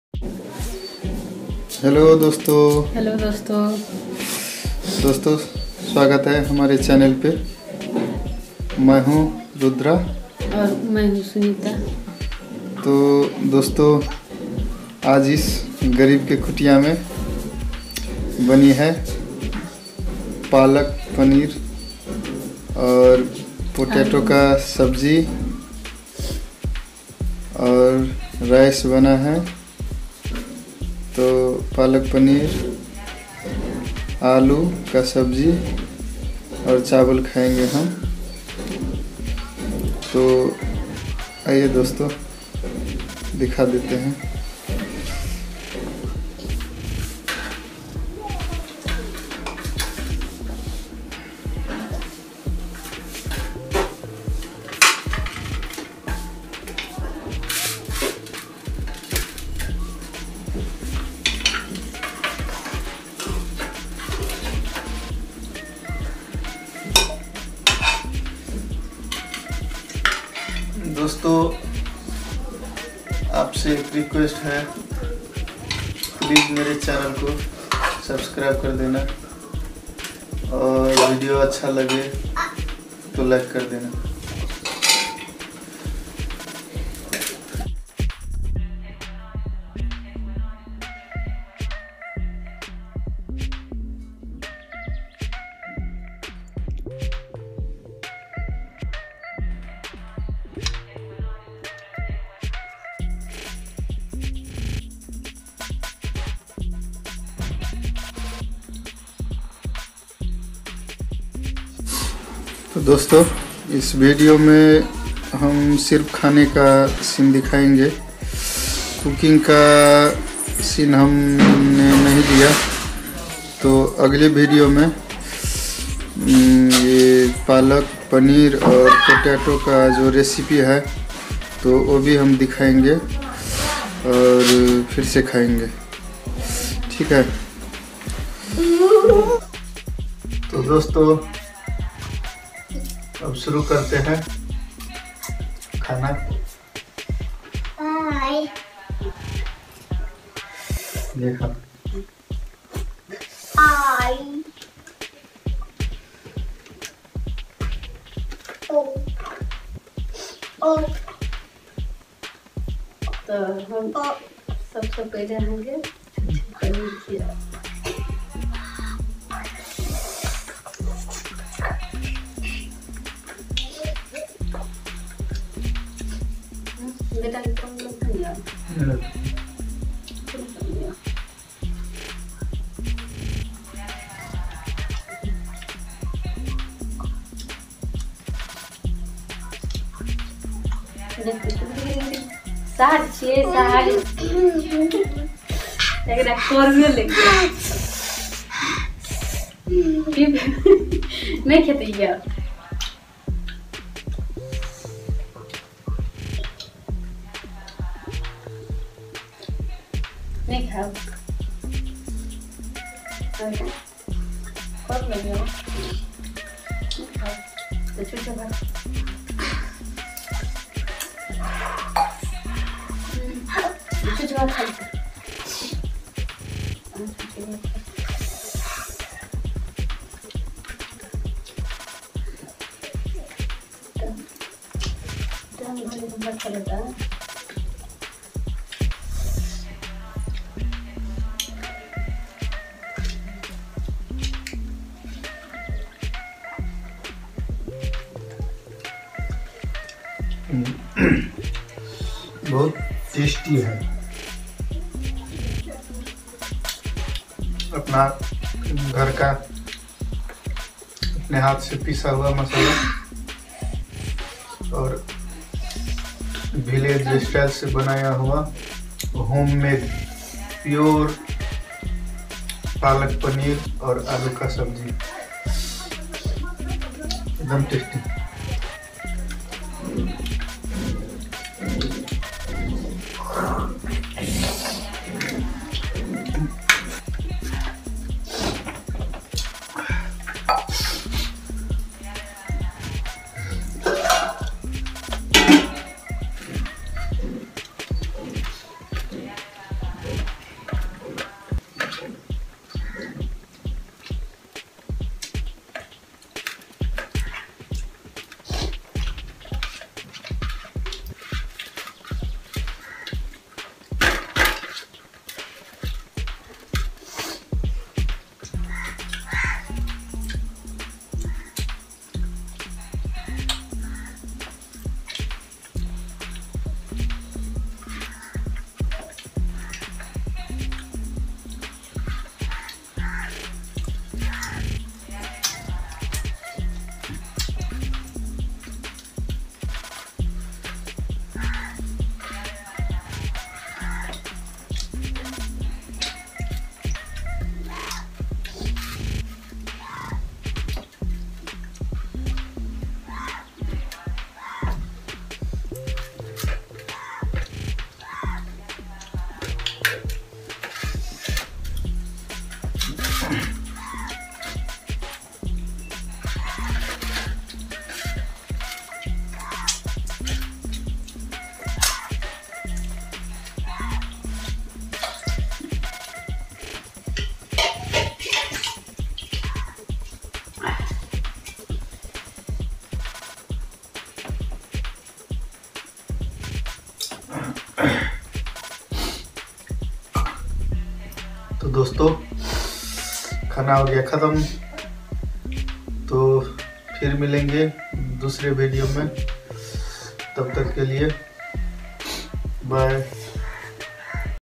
Husband wife romantic eating show part-11 | best palak paneer curry
In this video we have eaten palak paneer and potato curry with rice.